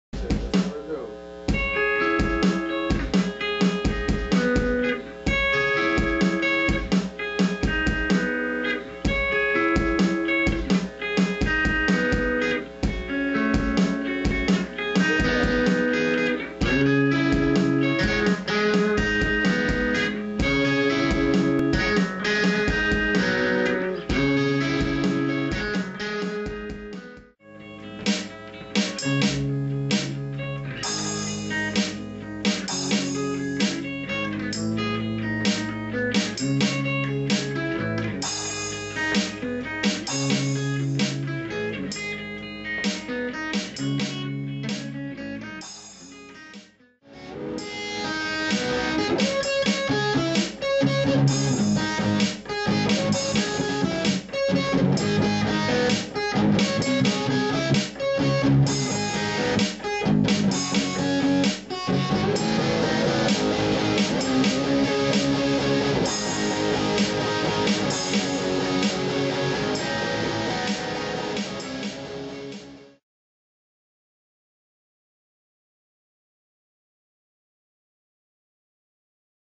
187 MUSIC LIVE SESSION
MUGZ OF LAWLESS AND RYAN WORKING ON NEW MATERIAL.